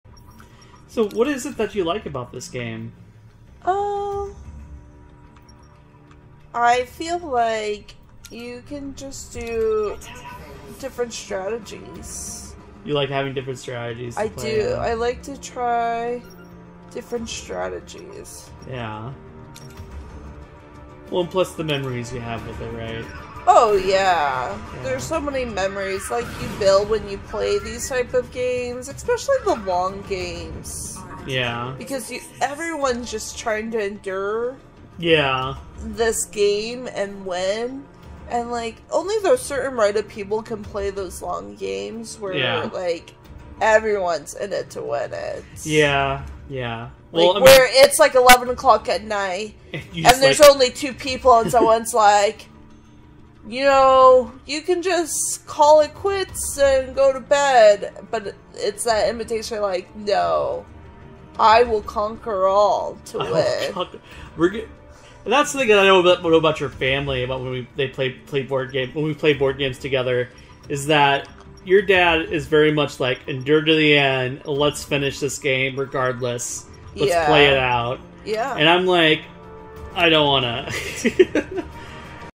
Risk | Risky Family Traditions
Jackie talks about some memories of playing board games.